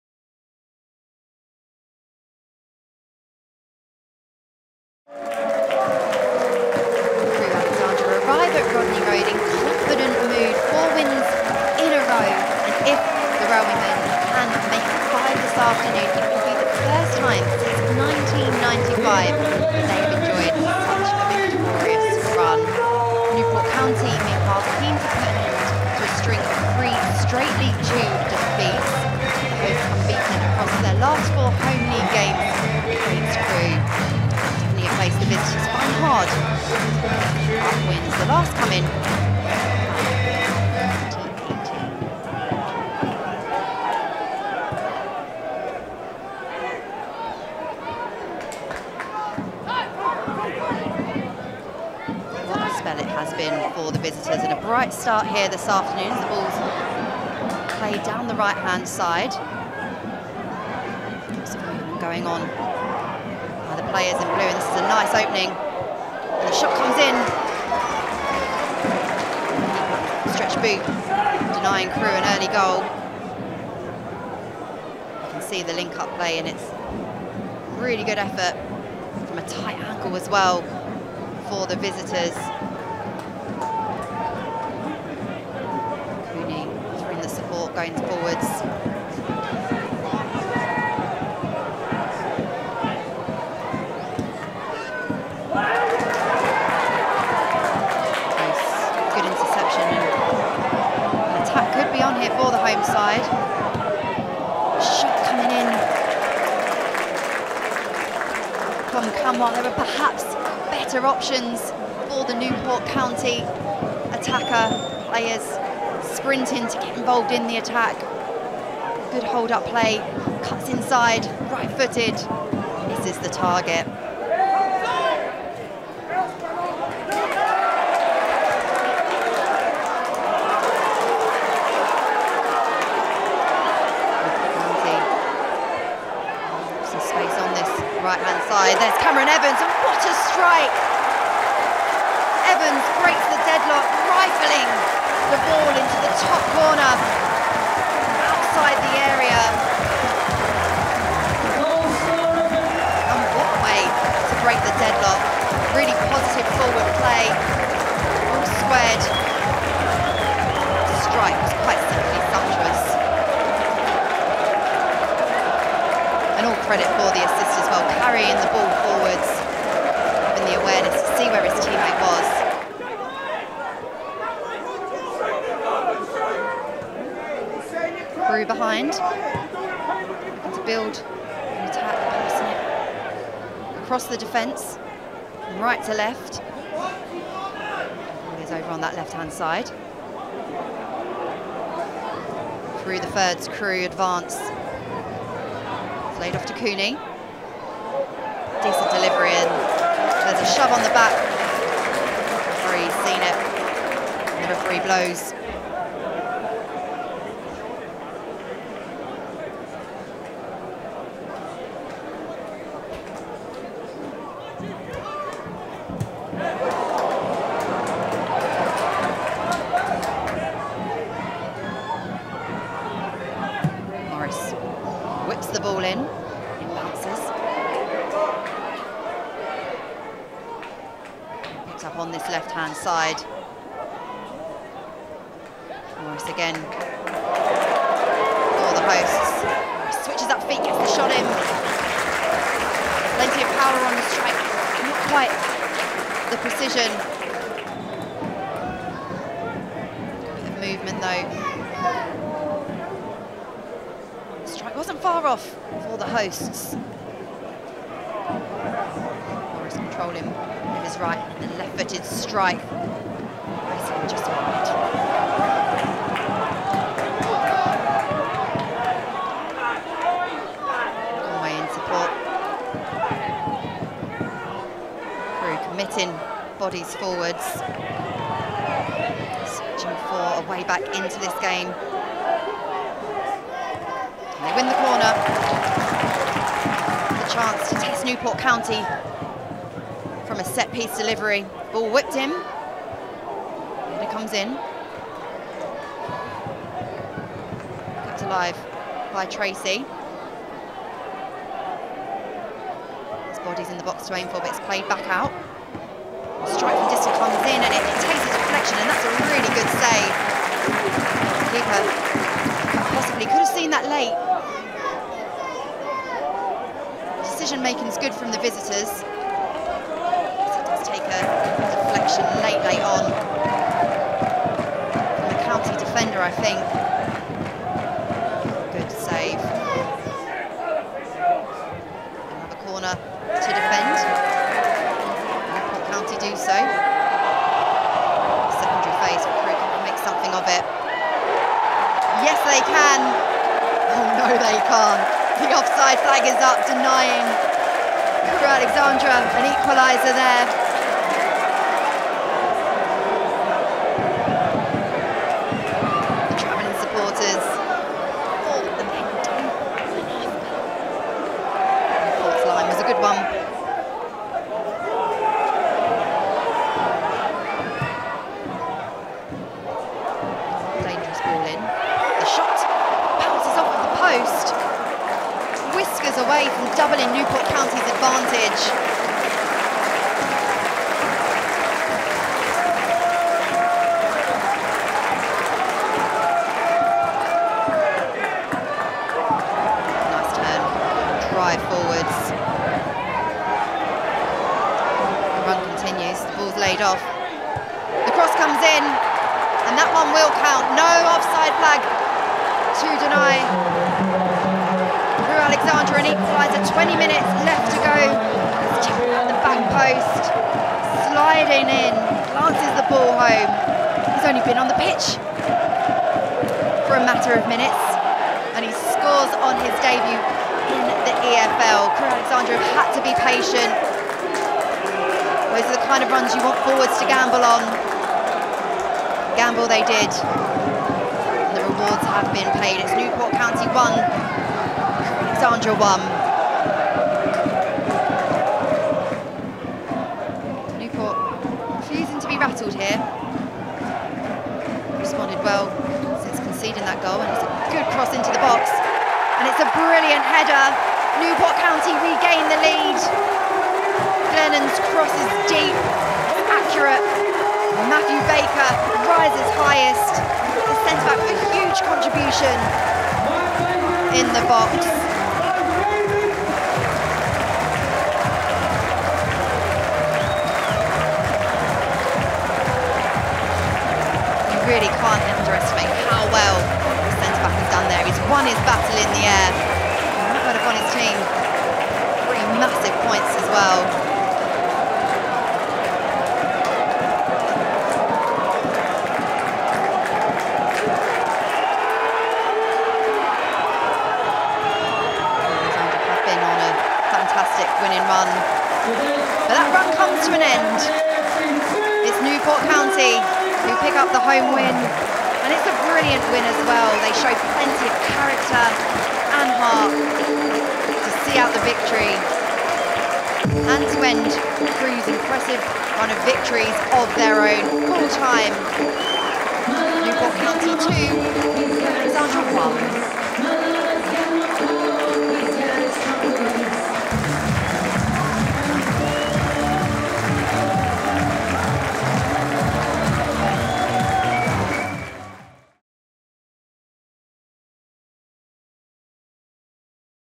EXTENDED HIGHLIGHTS | Newport County 2-1 Crewe Alexandra
Take a look back at extended highlights from Saturday's 2-1 win over Crewe Alexandra in League Two.